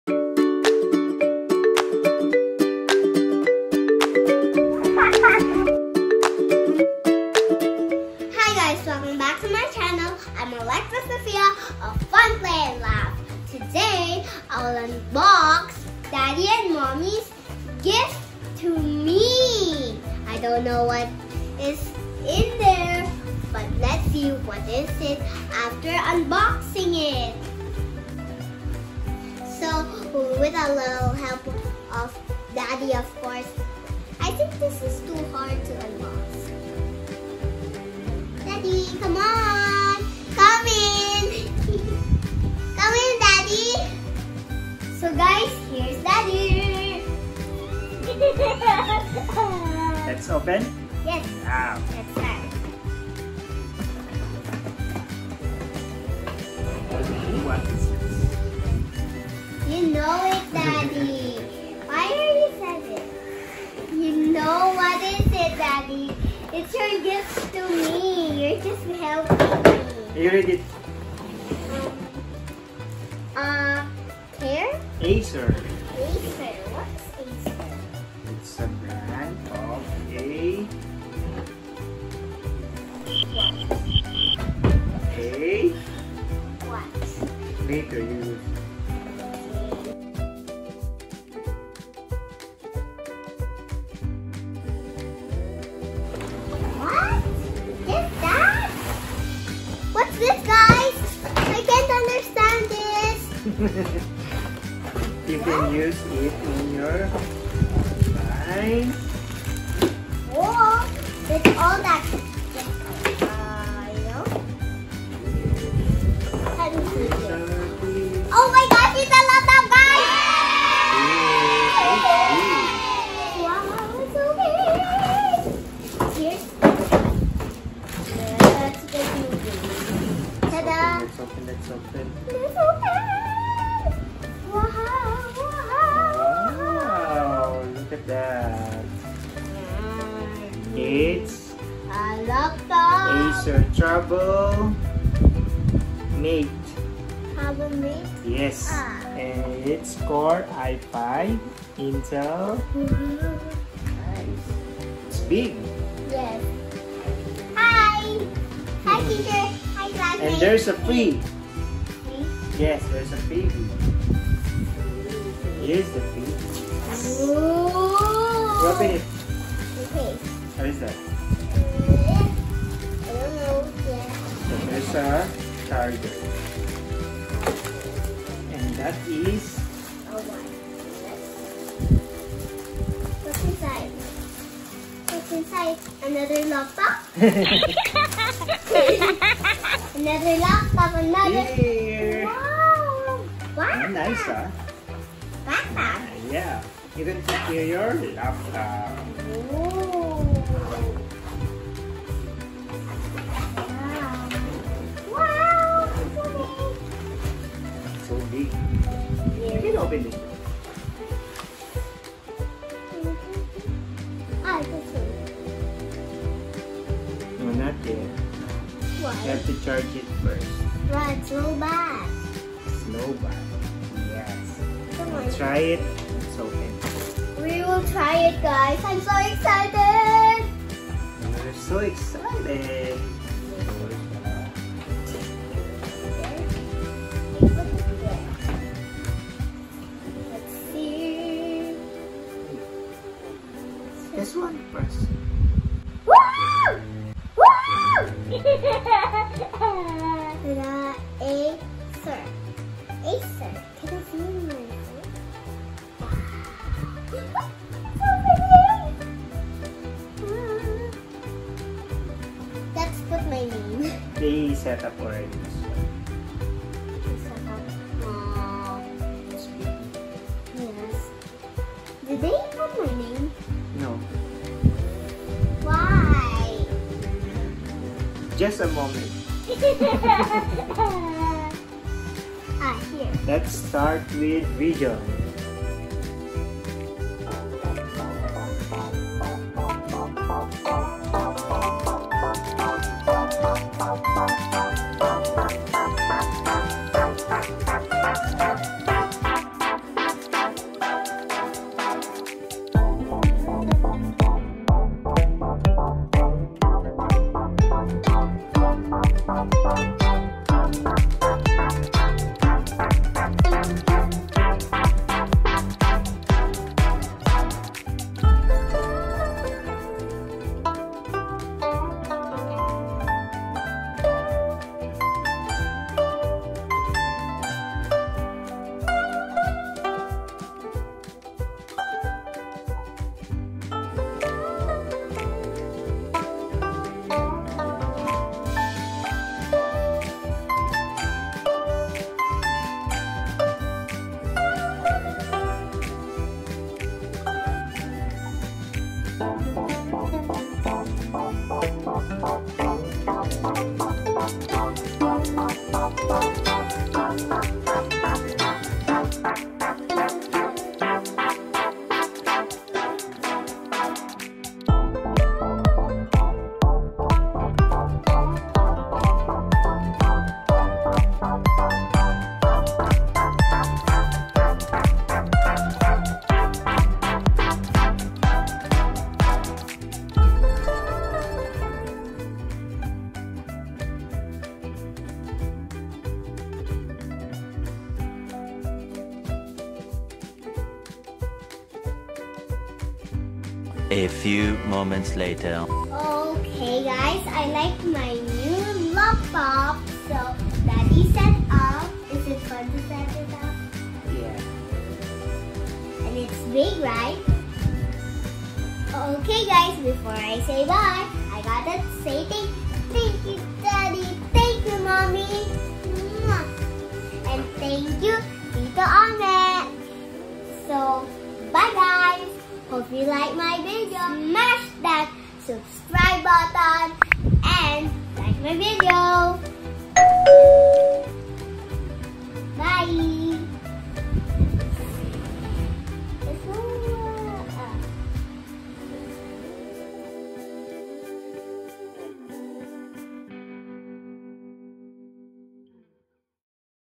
UNBOXING MY NEW ACER LAPTOP
Another unboxing video, but today's video Sophia will unbox her first personal Acer Laptop.

FB Page : Fun, Play n Laugh